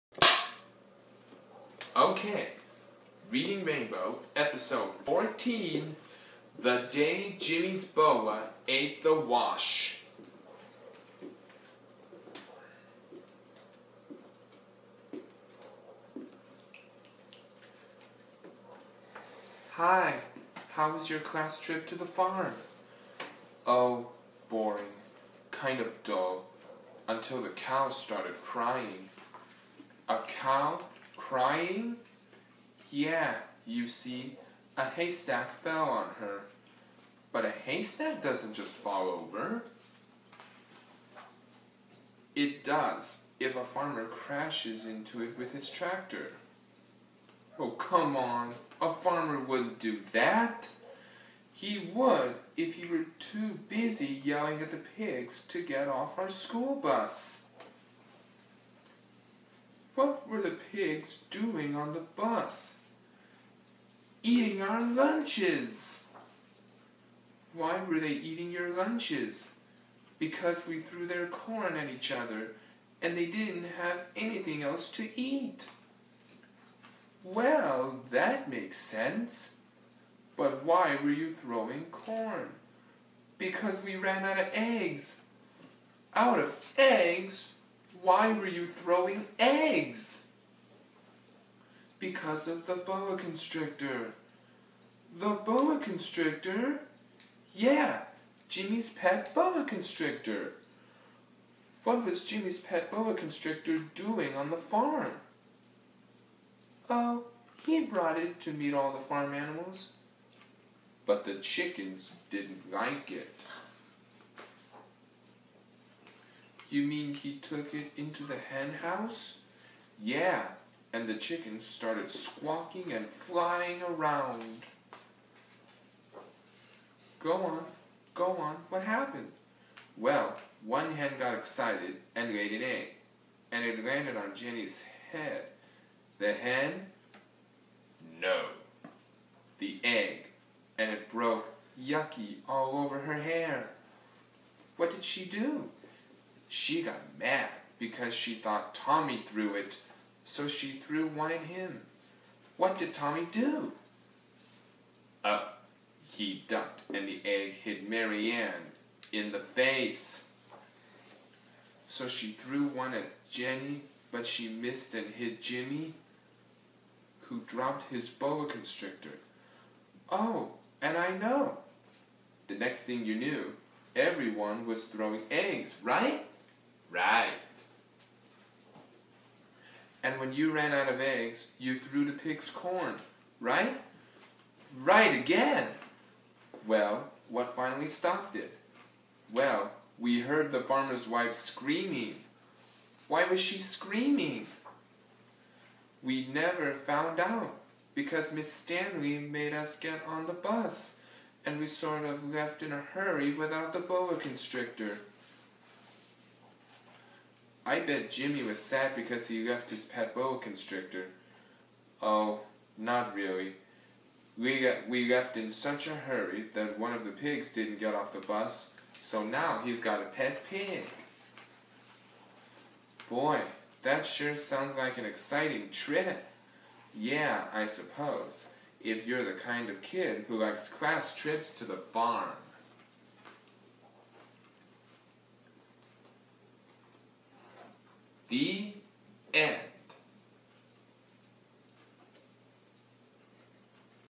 The Day Jimmy's Boa Ate The Wash
Jacob Reading a feature book from a Reading Rainbow episode